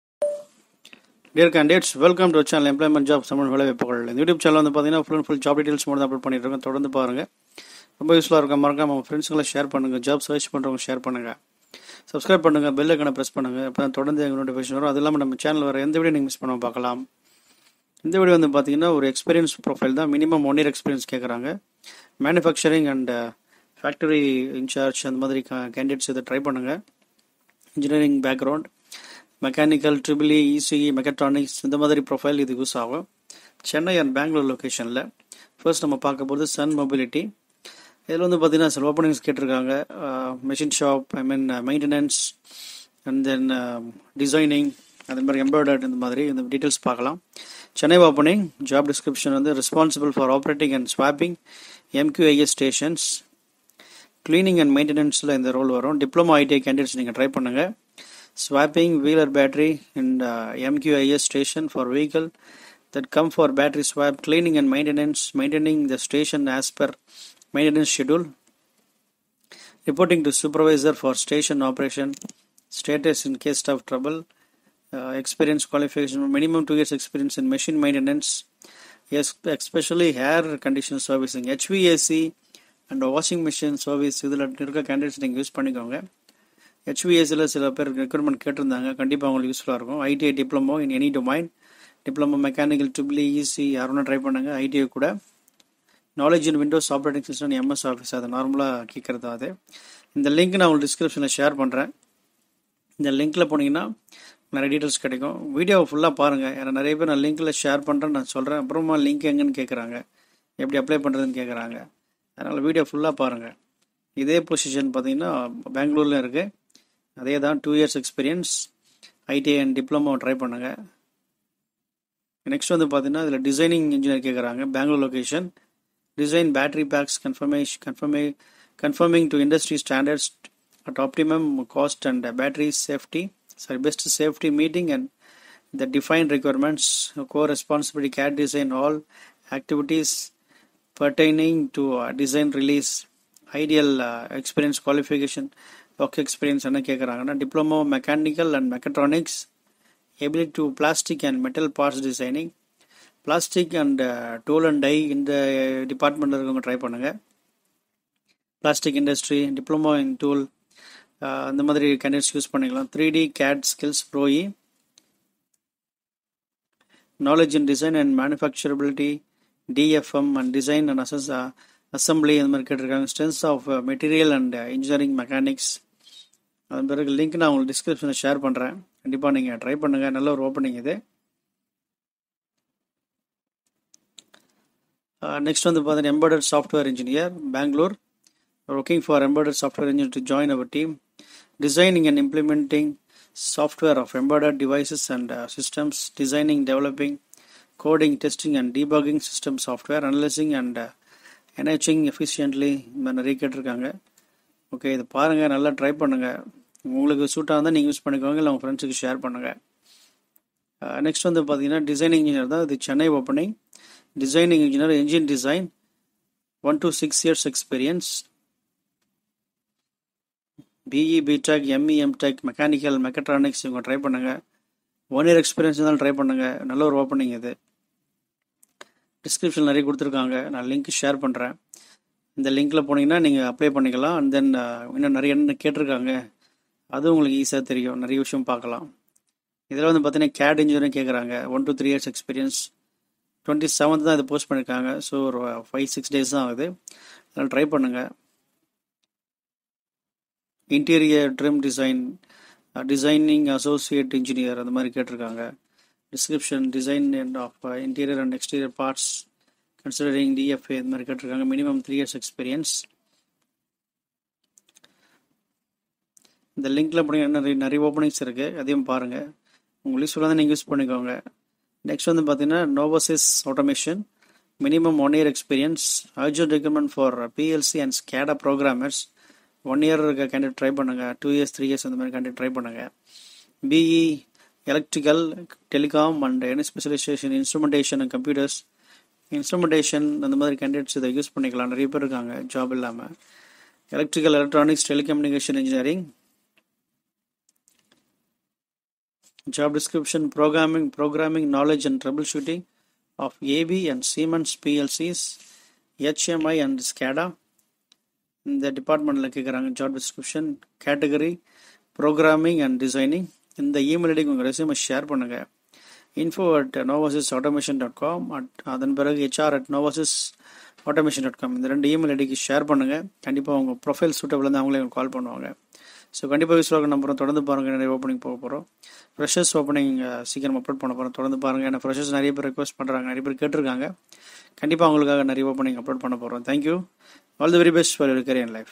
Engineering jobs design ,maintenance,embedded,plc scada 1 to 4 years exp
design engineer jobs#
embedded jobs#
maintenance engg jobs#

Station Operator - CHN - Batch 1
 Chennai, TN, India
Description
Role Description:
Responsible for Operating and swapping mQIS stations

Station Operator
Bengaluru, KA, India

Design Engineer
 Bengaluru, KA, India

Embedded Software Engineer ( TM )
Bengaluru, KA, India

Design Engineer - Engine Design
 Posted On:28-Aug-2019
Design Engineer
Exp 1 - 6 Yrs

CAE Engineer
 Posted On:27-Aug-2019

Interior Trim Design
 Posted On:27-Aug-2019
Design Associate Engineer

Novasys Automation
Urgent Requirements For PLC and Scada Programmers
1 - 4 yrs
Chennai, Hyderabad
Education-
UG:B.Tech/B.E. - Electrical, Electronics/Telecommunication, Any Specialization, Instrumentation, Computers
 Diploma - Electrical, Electronics/Telecommunication, Engineering

Job Description
Programming Knowledge and Troubleshooting of AB and Siemens PLCs, HMI and SCADA...
Industry: IT-Software, Software Services
Functional Area: IT Software - Application Programming , Maintenance
Role Category: Programming & Design